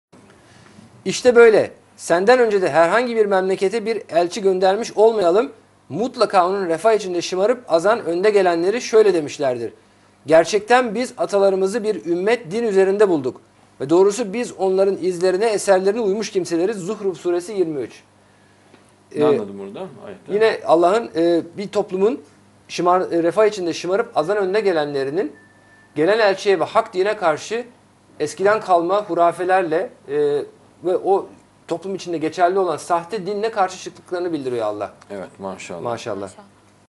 Zuhruf Suresi, 23. Ayetinin Tefsiri (5 Şubat 2010 tarihli sohbetten)...
Sayın Adnan Oktar'ın 5 Şubat 2010 tarihli röportajından Zuhruf Suresi ile ilgili açıklamalar.

OKTAR BABUNA: “İşte böyle, senden önce de (herhangi) bir memlekete bir elçi göndermiş olmayalım, mutlaka onun 'refah içinde şımarıp azan önde gelenleri' (şöyle) demişlerdir: "Gerçekten biz, atalarımızı bir ümmet (din) üzerinde bulduk ve doğrusu biz, onların izlerine (eserlerine) uymuş kimseleriz." (Zuhruf Suresi, 23).

ADNAN OKTAR: Ne anladın buradan, ayetten?

OKTAR BABUNA: Yine Allah’ın, bir toplumun refah içinde şımarıp azan önde gelenlerinin, gelen elçiye ve hak dine karşı eskiden kalma hurafelerle ve o toplum içinde geçerli olan sahte dinle karşı çıktıklarını bildiriyor Allah.

ADNAN OKTAR: Evet maşaAllah.

OKTAR BABUNA: MaşaAllah.